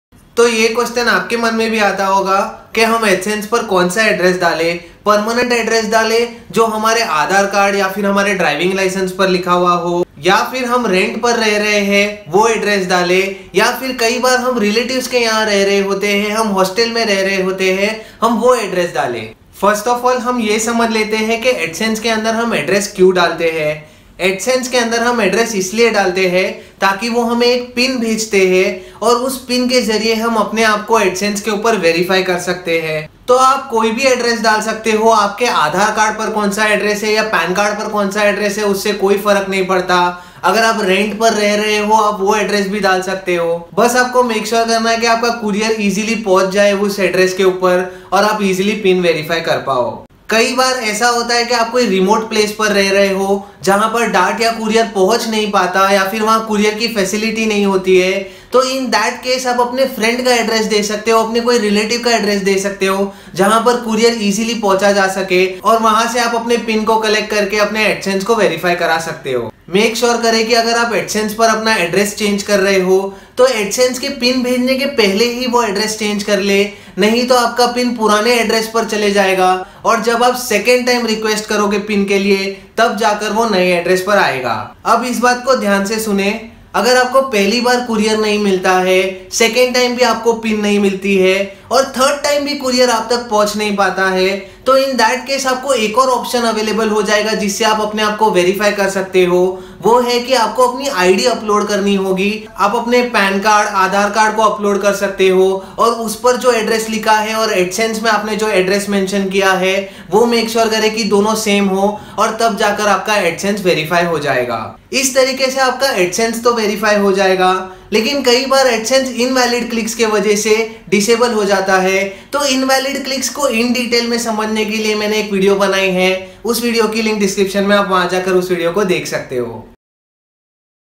Adsense Pin ke liye Address kaunsa dale ? Permanent Address or Rent Address or Relative Address
There is always a doubt in the mind whether we should give Permanent Address or Rent Address or Relative Address when we request for an Adsense Pin. Most of the time , young YouTubers are staying away from home in case they are studying , some stay in hostel or some stay at relative place. So is it ok to give Rent Address or Relative Address is what we will clarify in this video.

Also how to verify Adsense address in case you do not receive the Google Adsense Pin for 3 times is also covered in the video. Sometimes , we are staying at a place where is remote and the courier facility is not available and the Google Adsense Pin is not able to reach us , so in that case how to handle the situation is what we will discuss in the video.

Below is the video link to know how to protect Adsense from invalid clicks

Do go through our Creators Masterclass playlist that will help you grow your YouTube Channel and take it to the next level